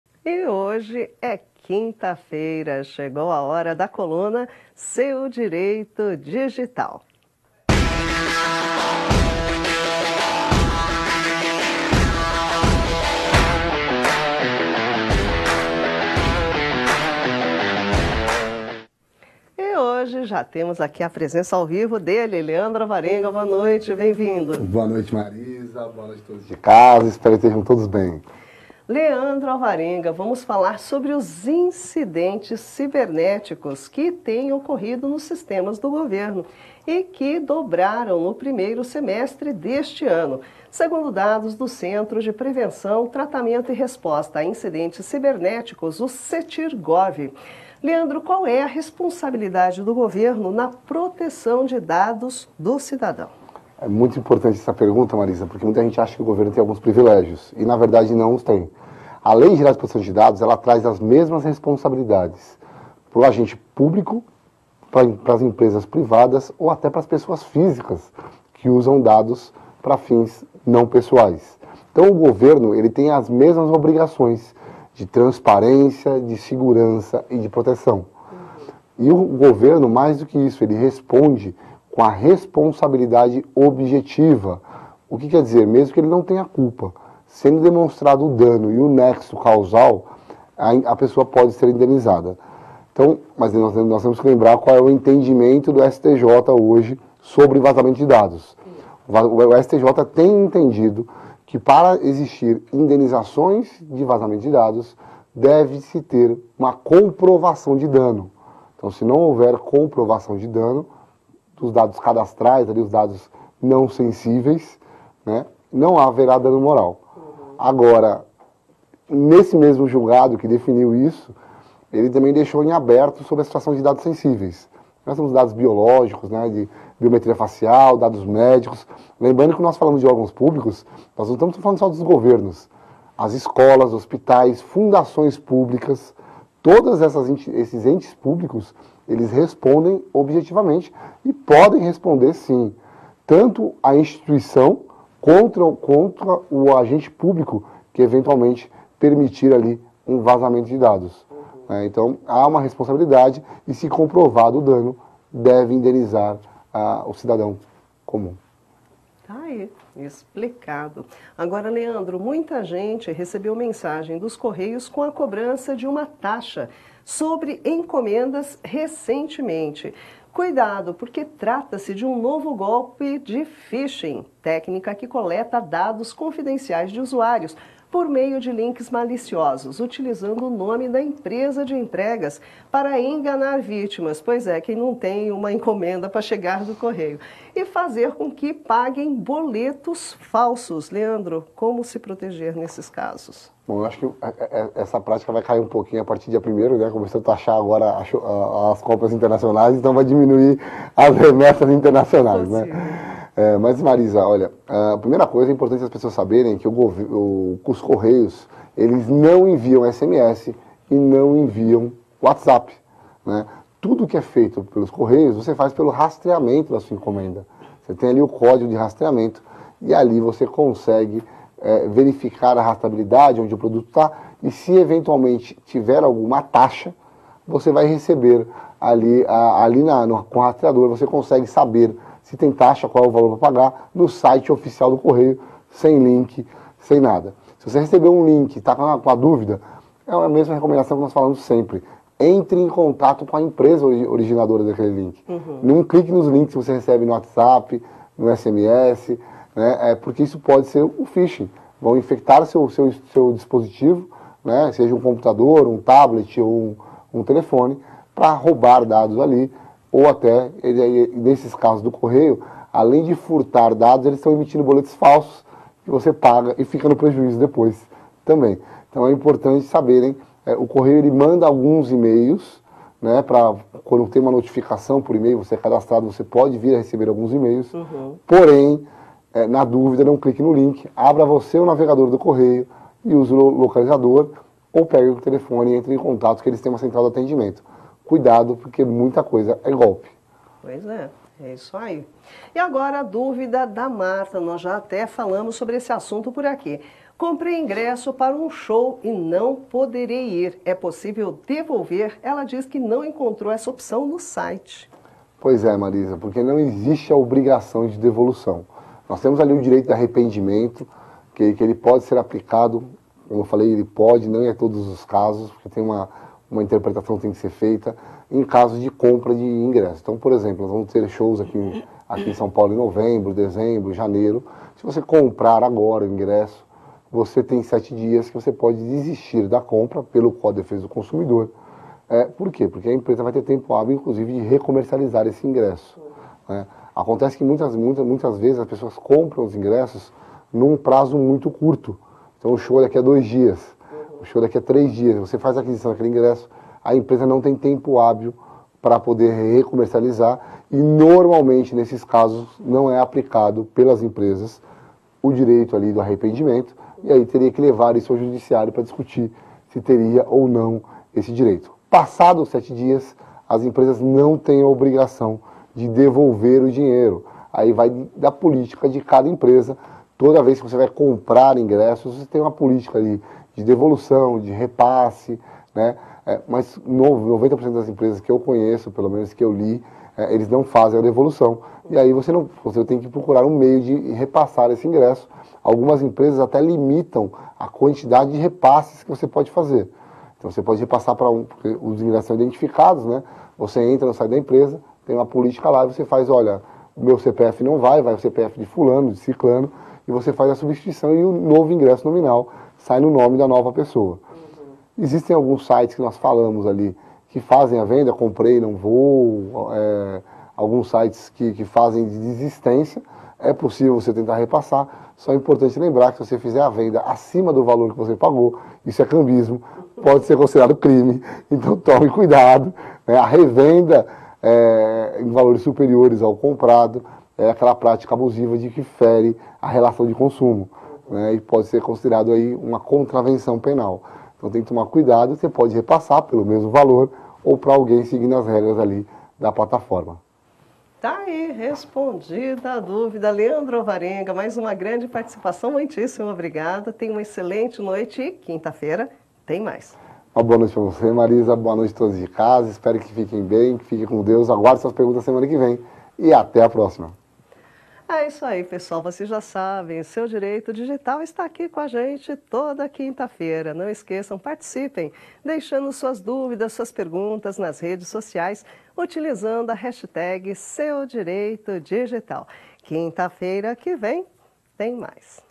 Seu Direito Digital: ataques cibernéticos ao governo estão mais comuns
Incidentes cibernéticos em sistemas do governo dobraram no primeiro semestre deste ano, segundo dados do Centro de Prevenção, Tratamento e Resposta a Incidentes Cibernéticos (CTIR-Gov). Qual é a responsabilidade do governo na proteção de dados do cidadão? Assunto para Leandro Alvarenga!